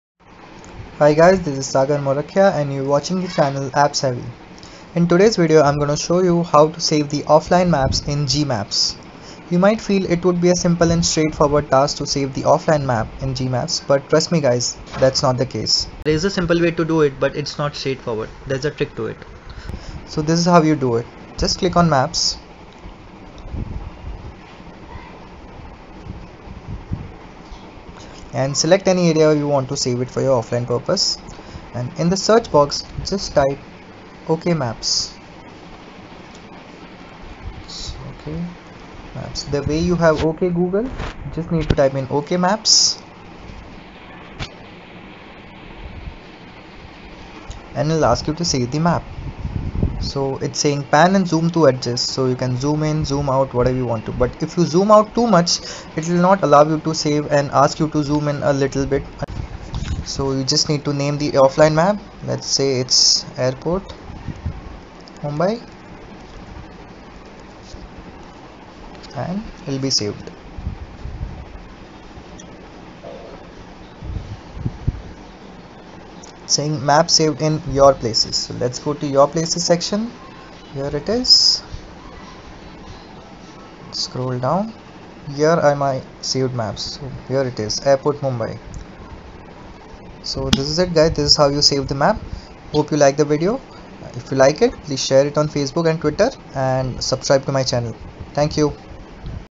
Save Offline Maps - OK MAPS
This video will tell you how to save the Offline maps in GMaps App.